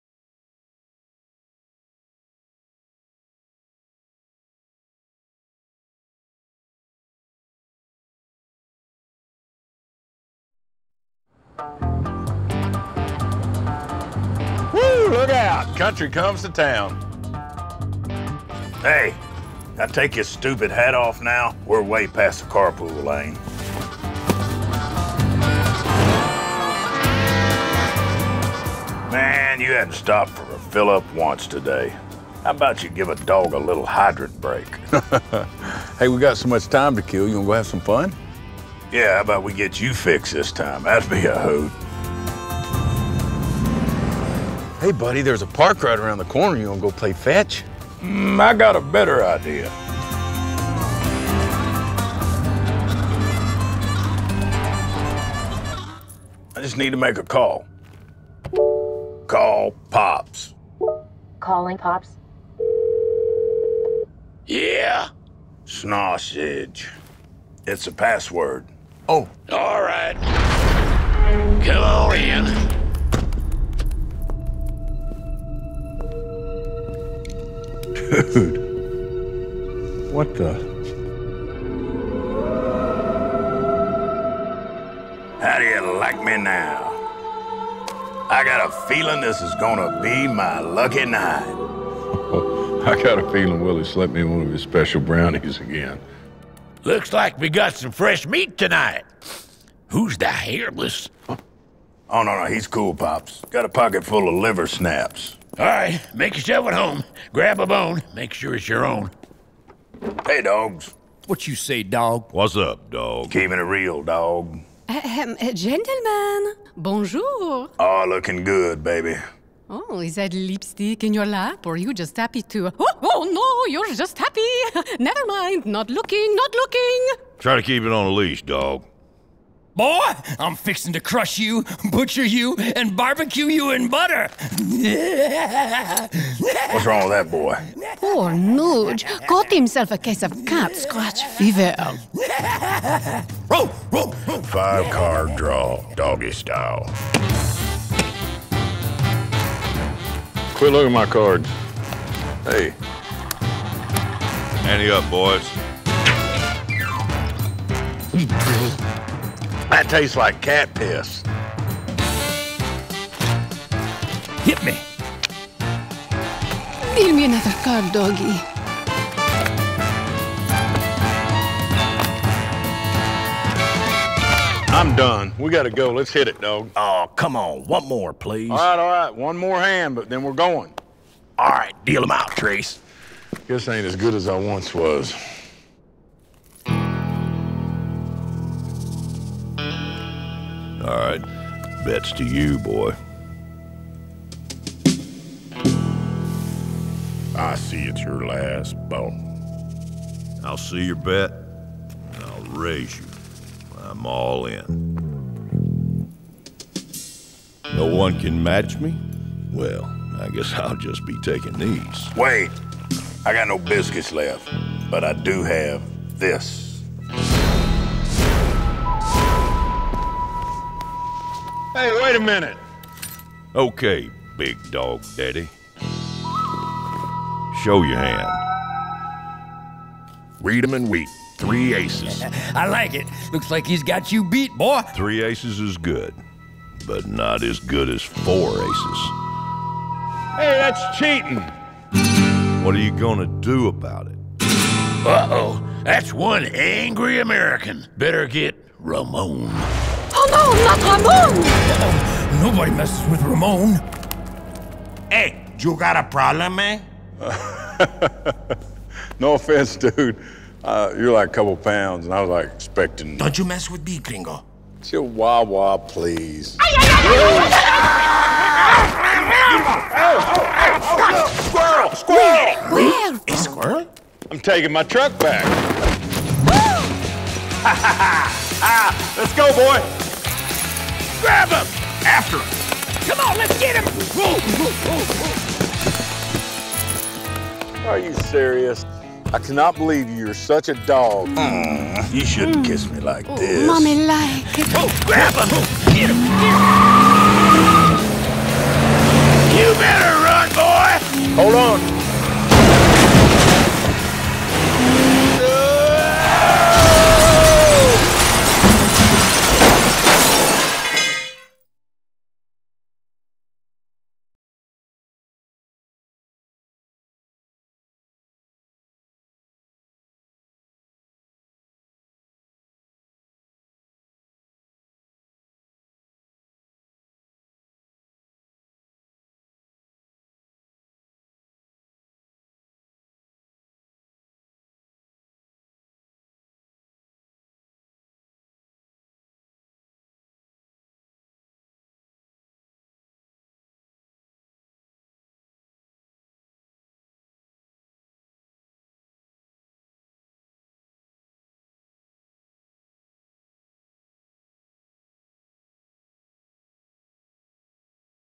2011 POKER NIGHT • Opening Concert Video Toby Keith tour Video starts at 00:11
This is the opening concert video used in Toby Keith's 2011 Tour. The idea of the video was to entertain the crowd, show off the F-150's features and bring out Toby Keith with a bang. We were asked to incorporate Toby's English bulldog and thought playing off the Dogs Playing Poker painting would be a fun way to go. Voice overs included Trace Atkins and Ted Nugent. As the video ends there was a loud explosion on stage along with pyrotechnics that would then reveal Toby and the band.

Please note, the video begins at the :11 mark.